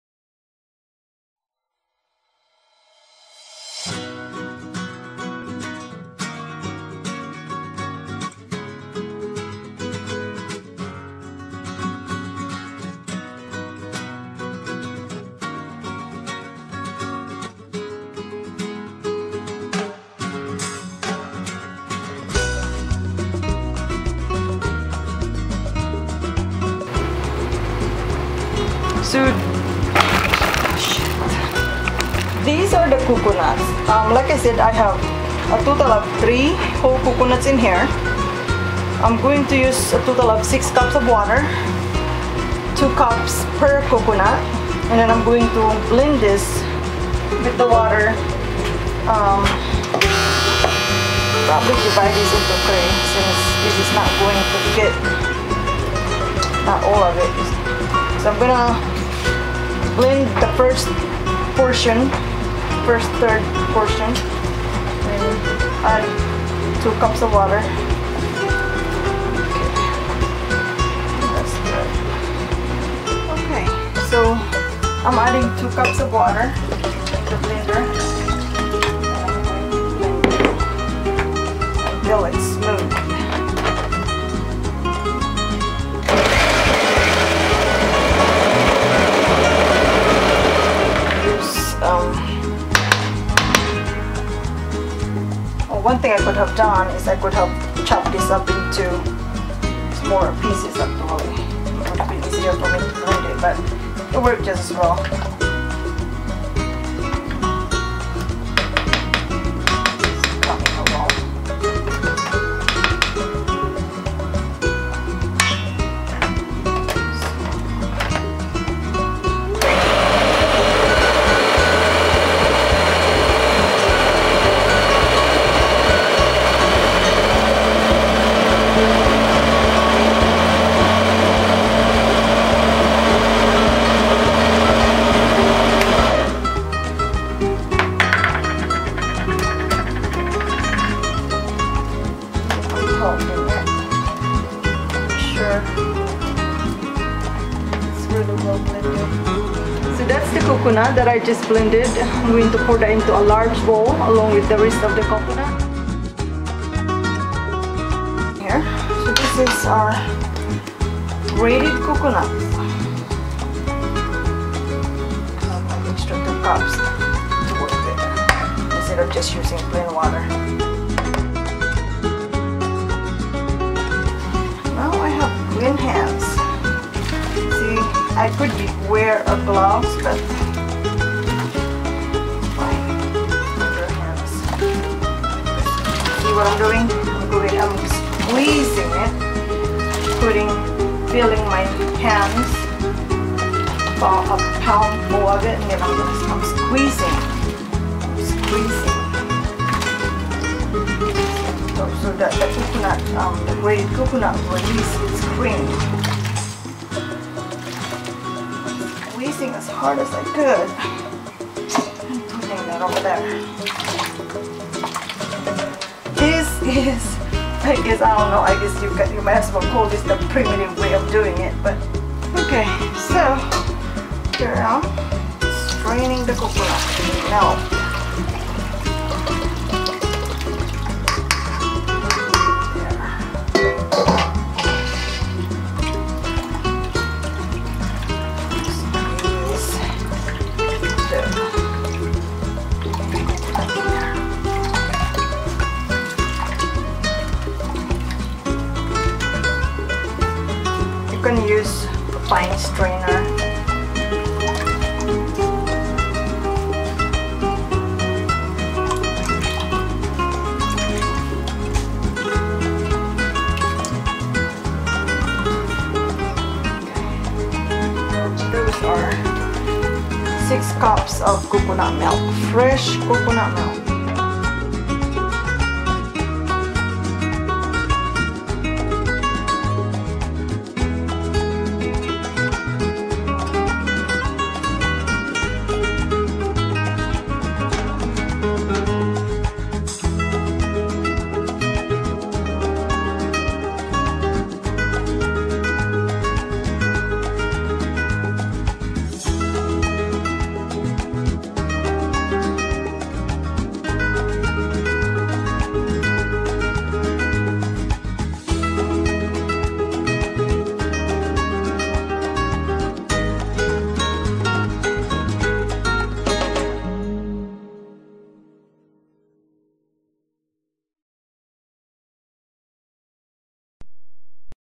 Squeeze Your Own Coconut Milk... fresh
fresh coconut milk... at home. excellent for baking, cooking, and penacolada. aromatic and great tasting.